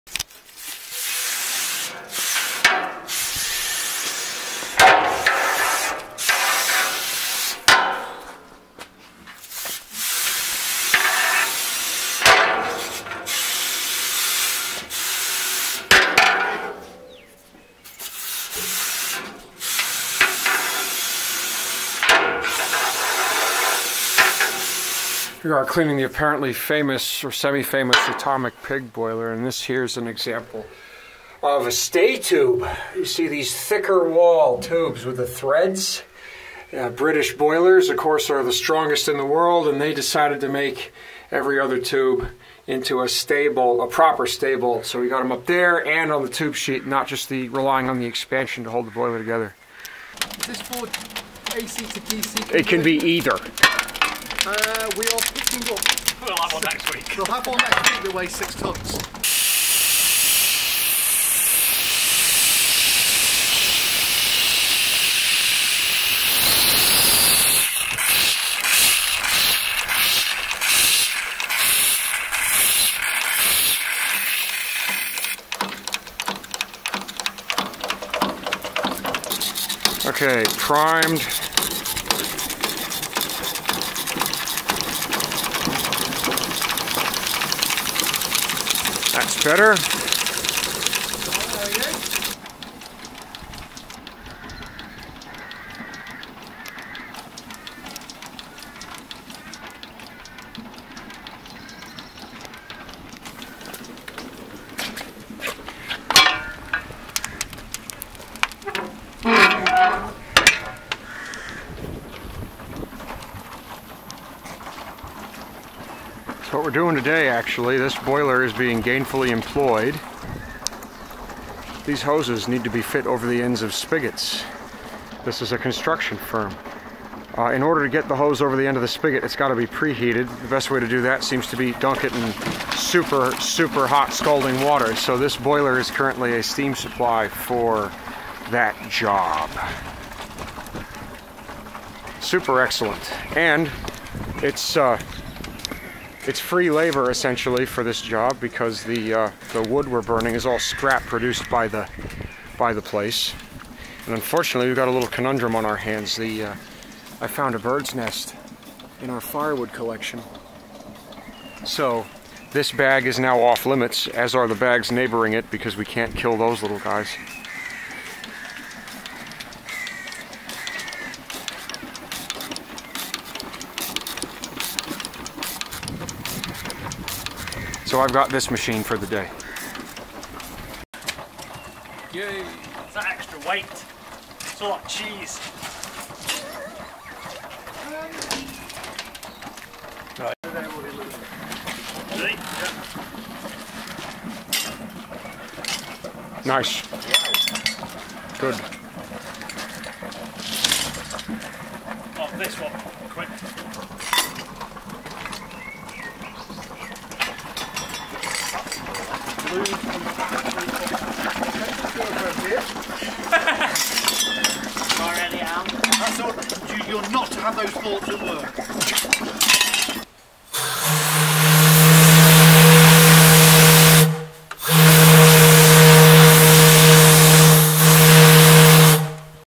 Gainful Employment for an Admiralty shipyard boiler
Because Great Britain is the way it is, the Atomic Pig boiler is a was a hand stoked coal fired portable boiler built by the Admiralty and used to provide shoreside auxiliary steam to nuclear submarines tied up at the pier side.  She now performs everyday work at a sewage company, to fit hose ends for the tanker trucks.  There will come another video as later we are due to run a high speed steam engine with her and make quite a lot of DC power.

I have always said heritage is a misnomer and does not actually exist, because these machines are still the machines they were when built and the laws of physics have not changed since then, so understand them, use them and care for them as you would have back then.  You will find they are more useful than the garbage built today.